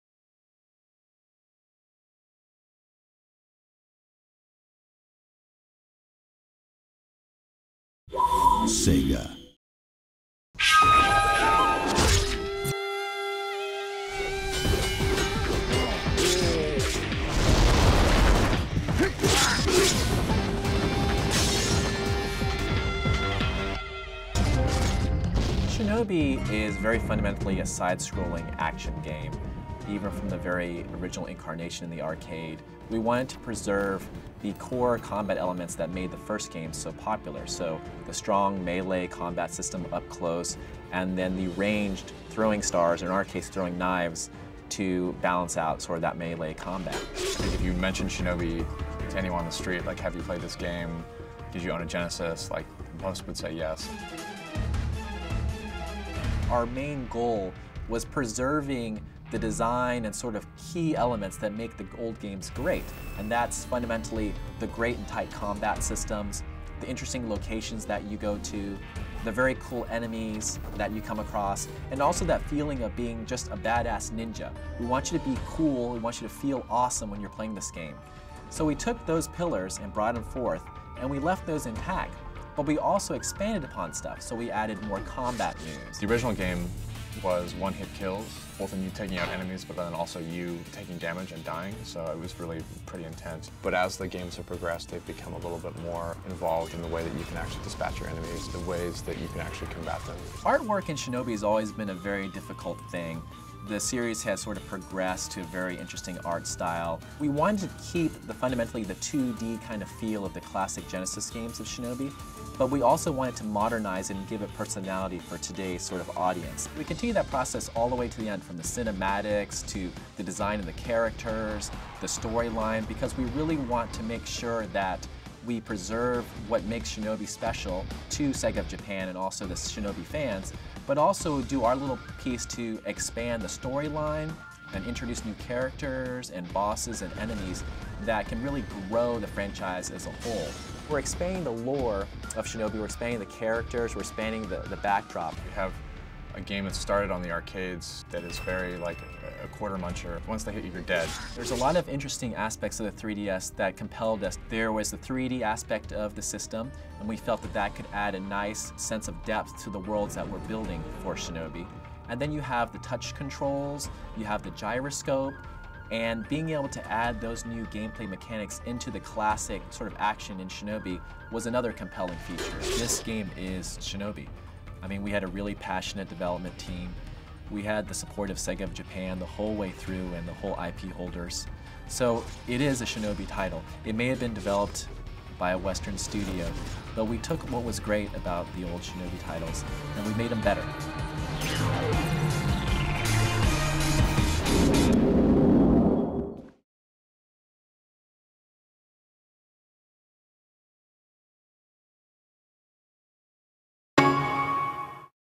Shinobi 3DS - Behind the Scenes - History
Take a look behind the making of Shinobi on the 3DS with interviews from our producer Stephen Frost and community manager Julian Mehlfeld.  First video features the history of Shinobi and the makings of a true Ninja game, from past, to future.